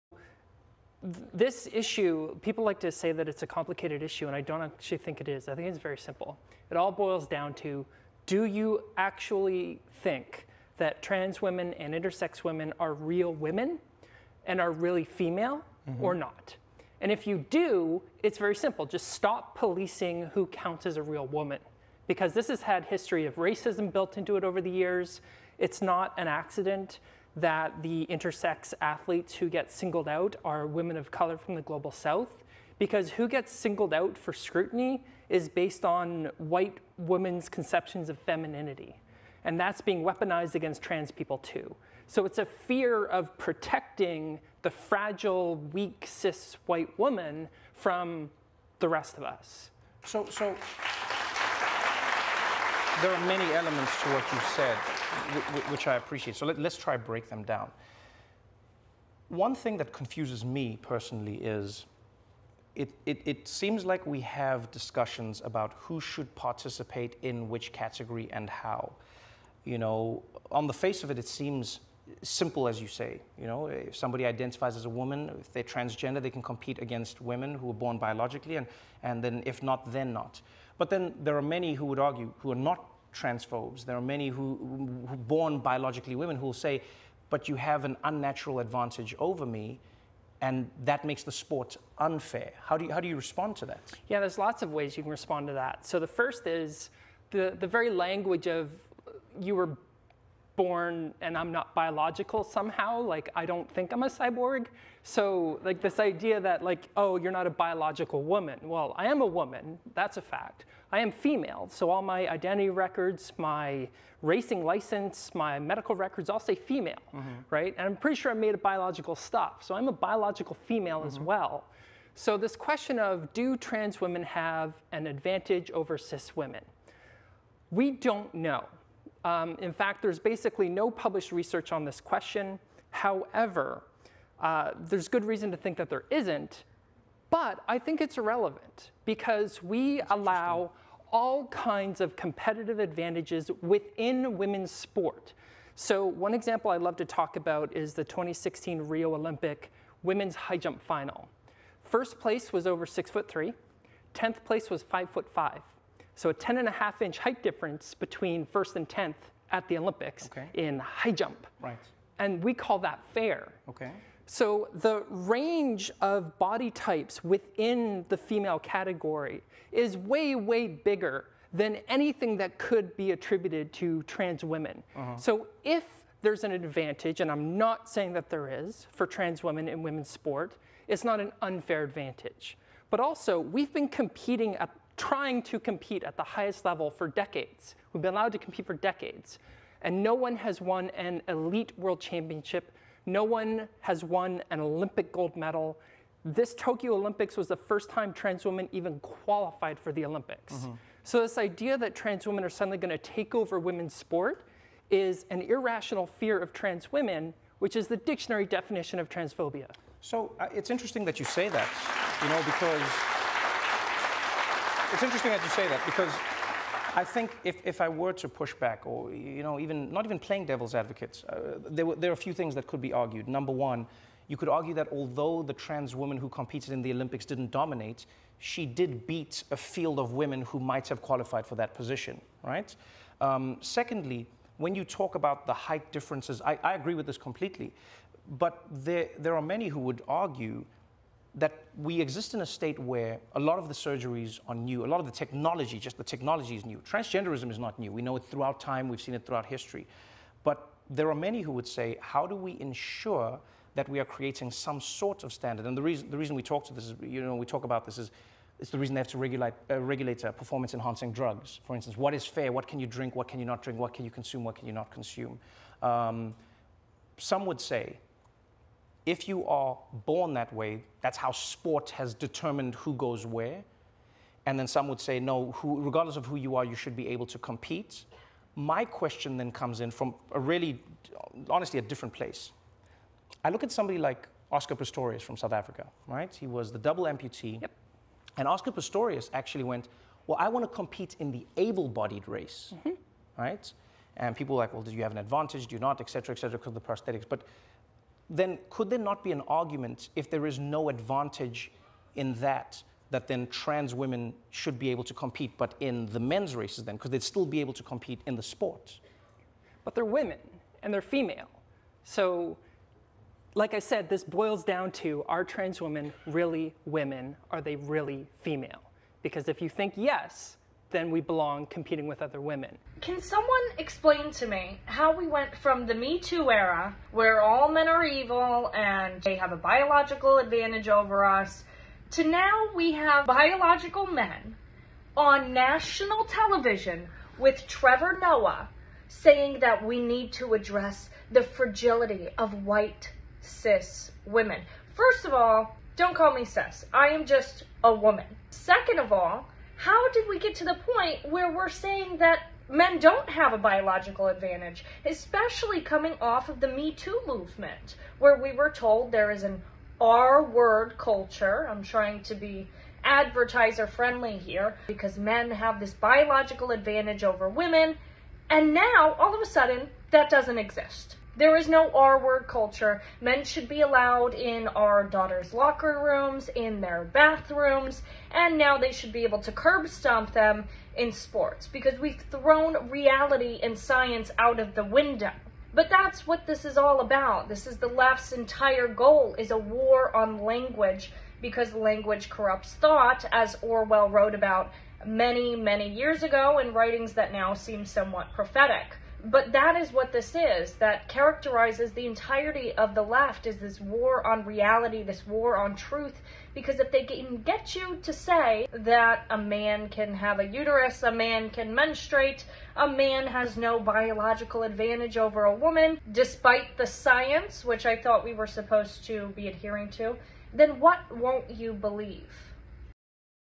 Activist Says Girls Who Don't Want to Compete Against Trans Athletes are "Weak" 🤡
Trans cyclist "Veronica Ivy" just went on Trevor Noah and made every logical fallacy known to mankind, then insulted women and accused any critics of being racist misogynists to boot! Ashley St. Clair responds.